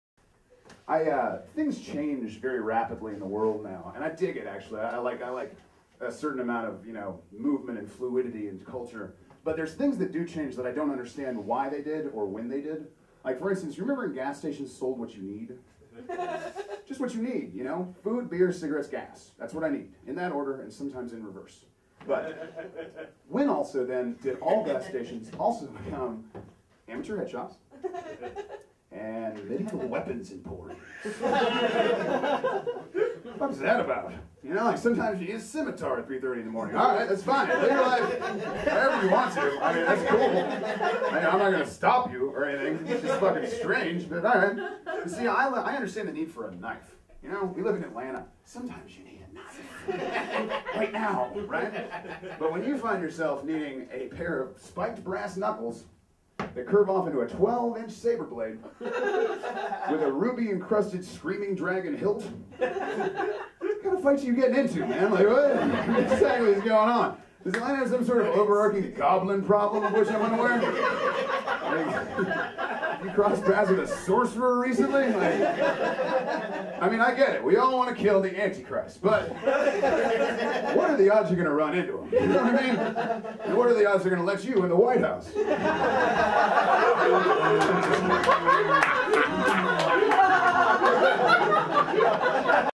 Gas Stations
Evan Fowler at The Basement Theater in Atlanta, GA. 2010